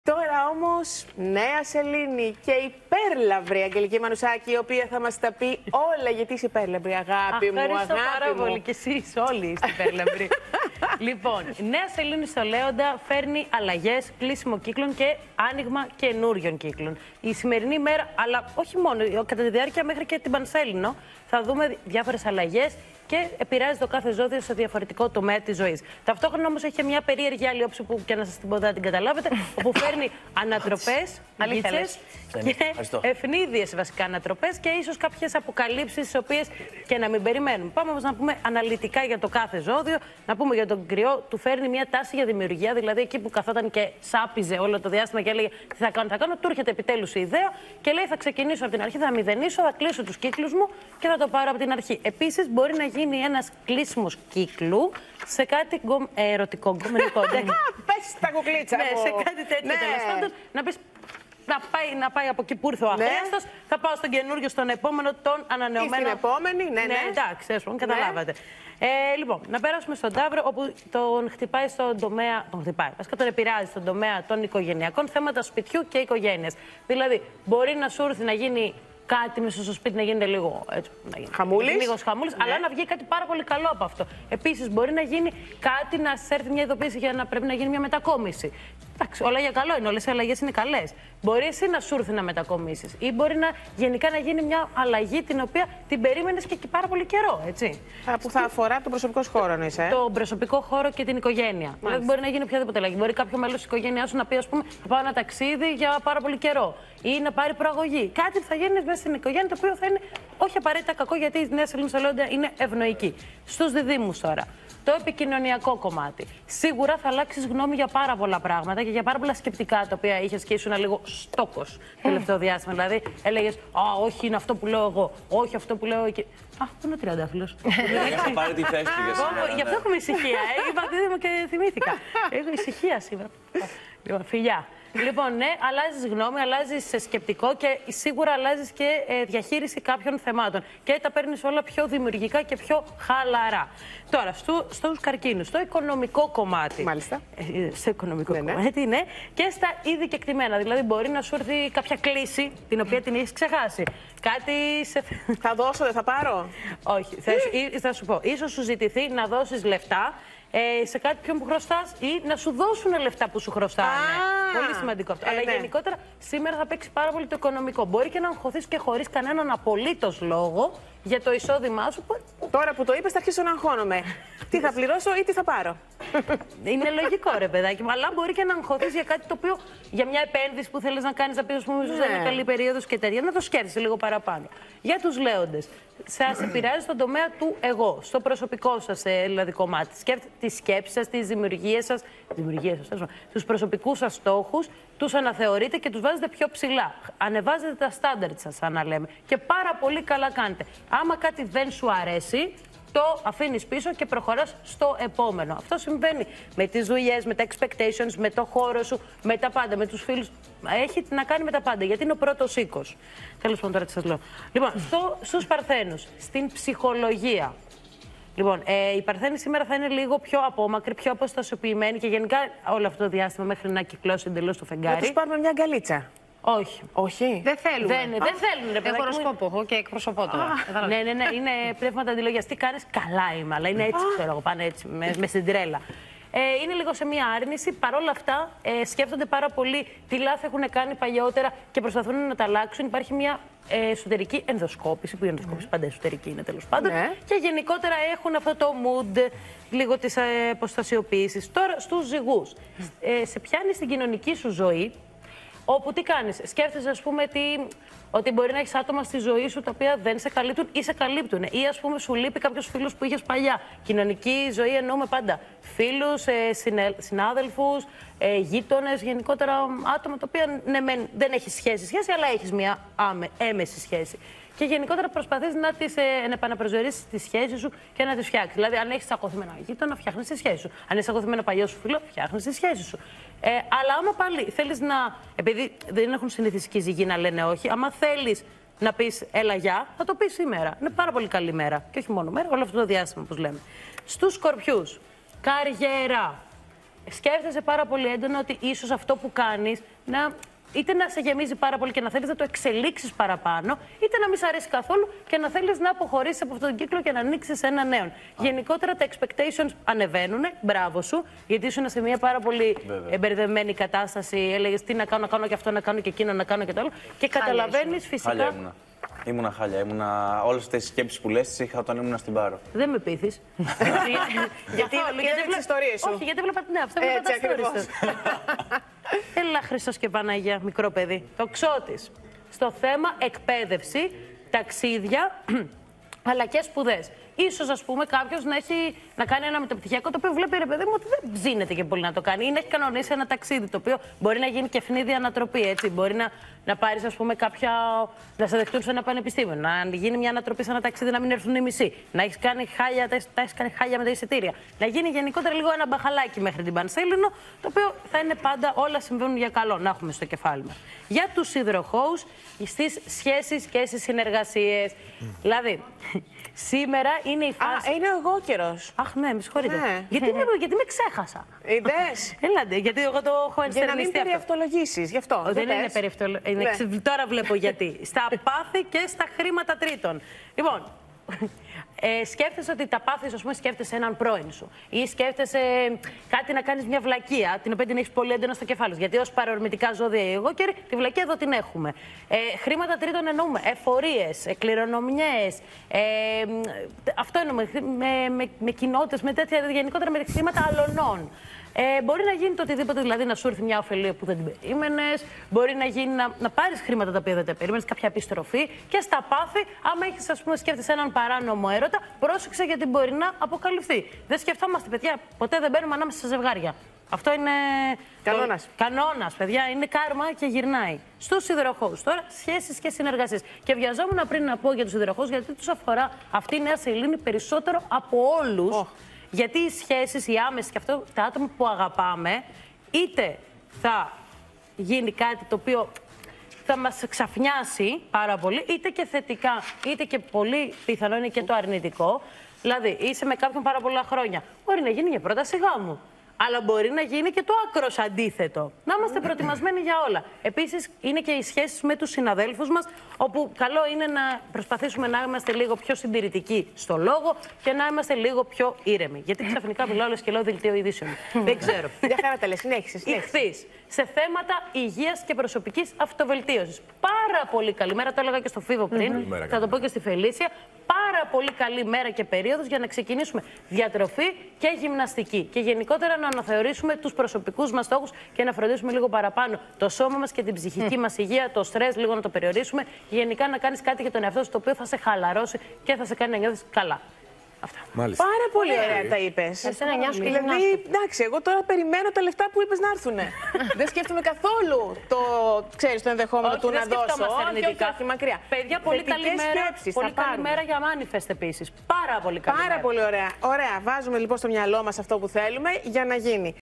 Ζώδια: Νέα Σελήνη στο Λέοντα – Φέρνει ξαφνικές αλλαγές και στο προσκήνιο παλιούς έρωτες | OPEN TV
Γίνετε μέλος στο κανάλι του OPEN για να αποκτήσετε πρόσβαση σε προνόμια:
Οι αστρολογικές προβλέψεις της ημέρας από την Αγγελική Μανουσάκη.